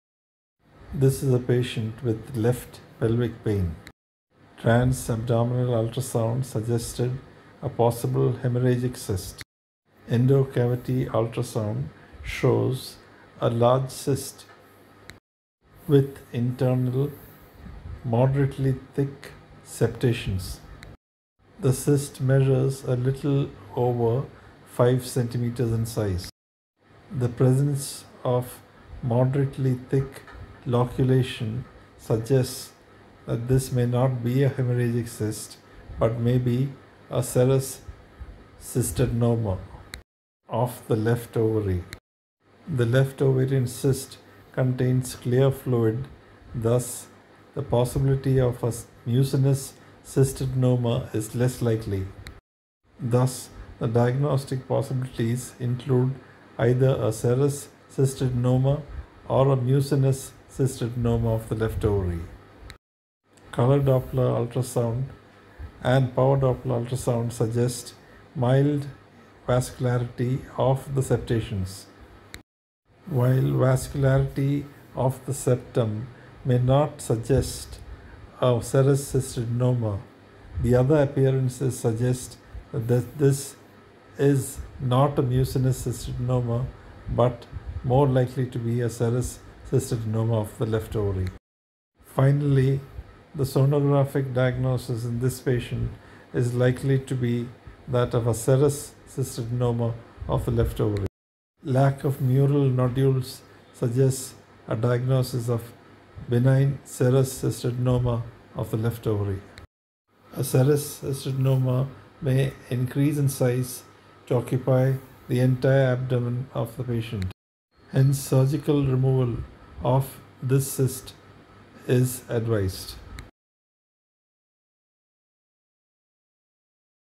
Serous vs mucinous cystadenoma left ovary. Left ovarian septate cyst ultrasound and color Doppler
Complex ovarian cysts on ultrasound may create a diagnostic dilemma. This complex left ovarian cyst has 3 differential diagnosis:
hemorrhagic cyst vs serous cystadenoma vs mucinous cystadenoma.